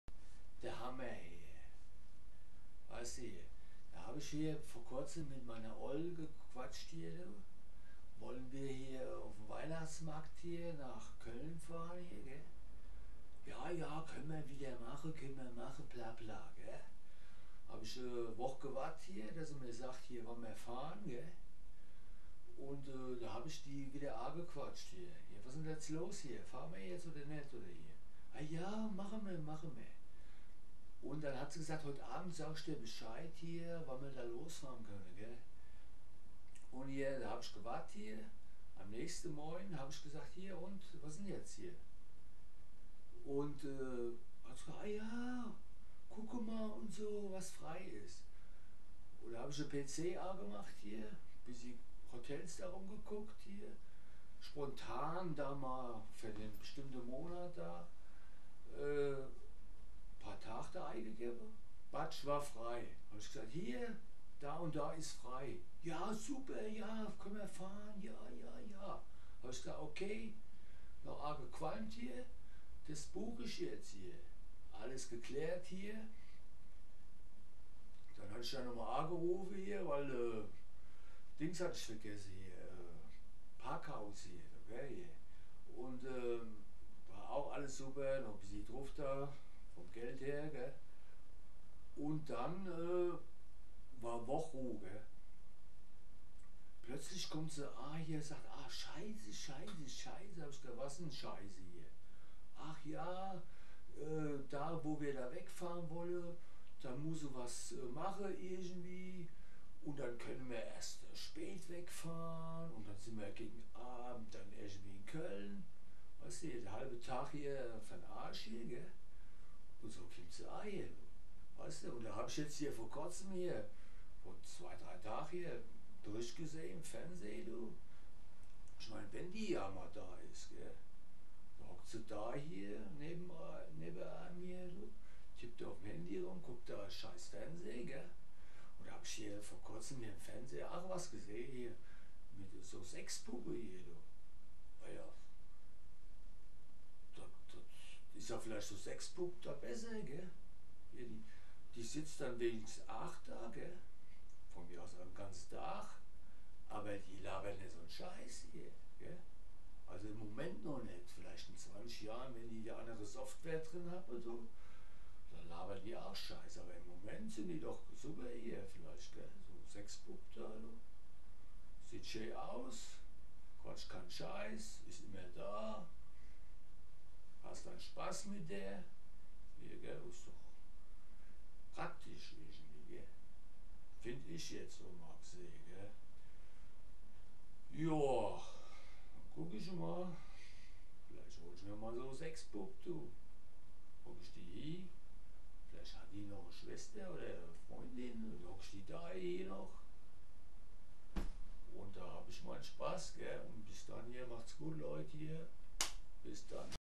Sexpuppe ja oder nein?Sex
Echte Frau oder besser Sexpuppe?Sex Ich wollte auf den Weihnachtsmarkt Köln 2017 mit einer Frau.Macht das Leben
mehr Spaß mit einer echten Frau oder mit einer Sexpuppe?Künstliche Intelligenz Lovedoll Lovedolls Sex Gummipuppe Sexpuppe Echte Frau#Love